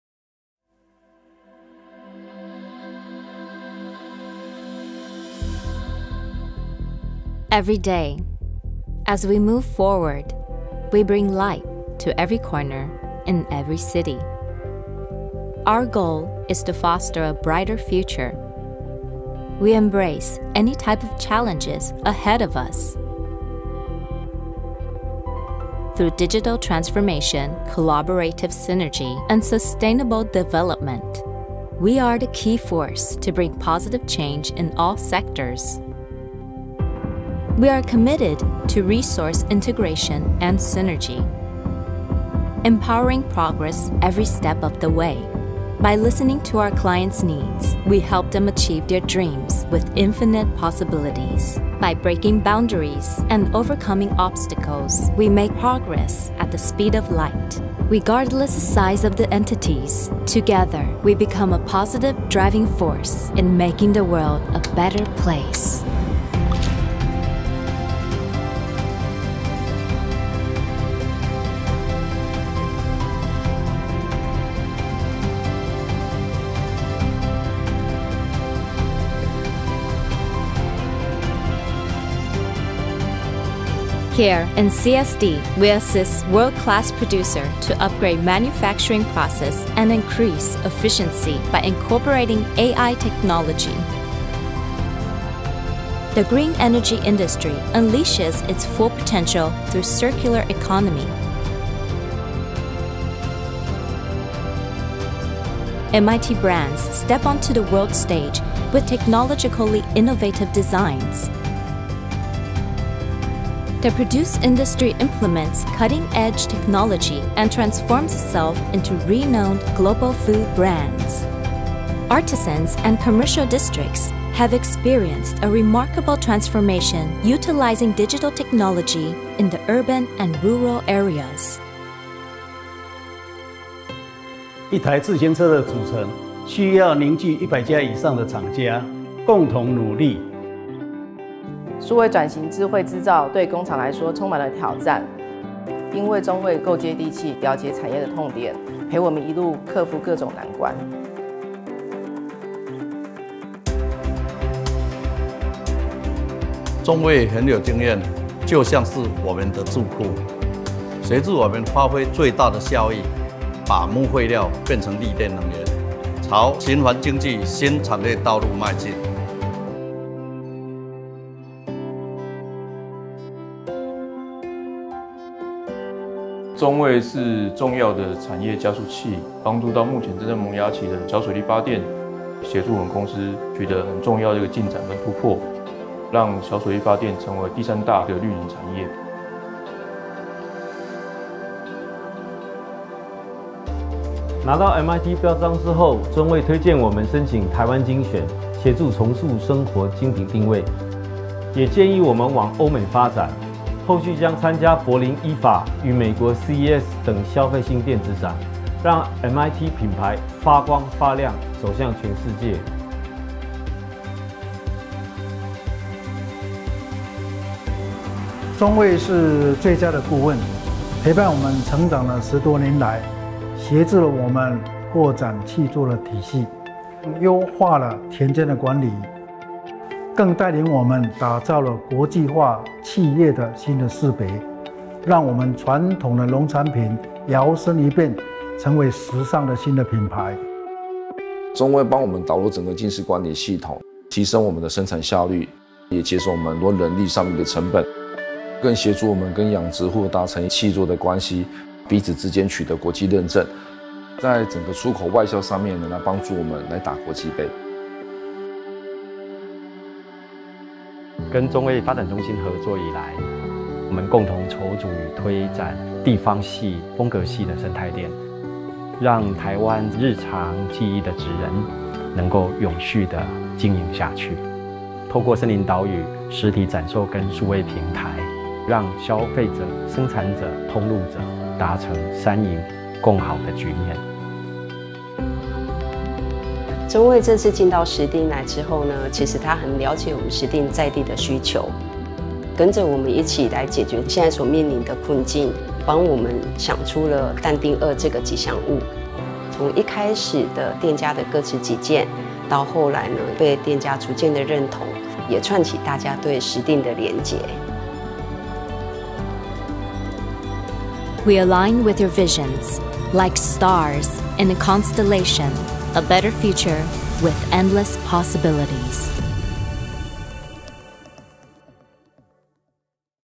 Corporate Synergy Development Center Publicity Film - Corporate Video
In the 1980s, Taiwan was on the verge of changing its industrial economy into a high-tech one. It was critical to improve manufacturing and marketing collaboration, as well as international competitiveness. To assist in responding to the competitive pressures of globalization and liberalization, as well as to speed up the upgrading of domestic industries. The Bureau of Industry, Ministry of Economic Affairs, formed the Central Satellite Factory System Promotion Team on July 1, 1984. To make it more versatile and serve more industry players, the institution was reorganized as the "Corporate Synergy Development Center" in 1990.